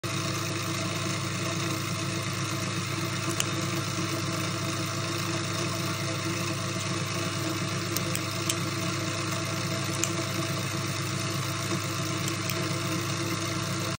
This Medford Smooth Criminal is an incredible Button Lock Knife
This Medford Smooth Criminal is a great Button Lock edc Also, Here Is a Stratocaster Pickup being Made